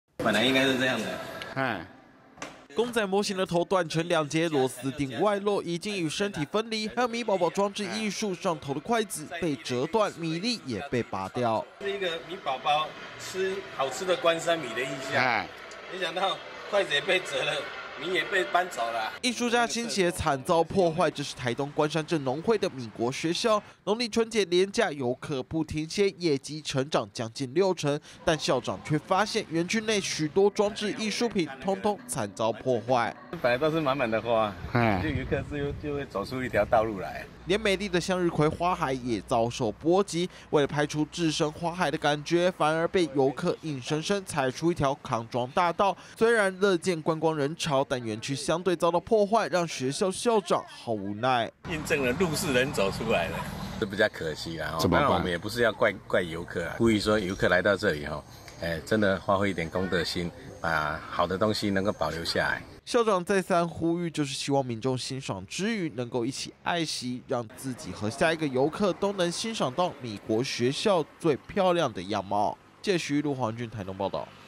20190214中天新聞 米國學校春節夯 裝置藝術卻遭斷頭破壞
關山鎮農會的米國學校，是往返花東縱谷必經之地，尤其今年農曆春節，大受遊客歡迎，業績成長了將近6成，但農會人員在欣喜之餘，卻也發現園區內，多件裝置藝術品慘遭破壞，有的被斷頭，有的零件被拔走，甚至連向日葵花田，都被踩出一條路，學校校長趕緊跳出來呼籲，希望大家欣賞之餘，能保持公德心。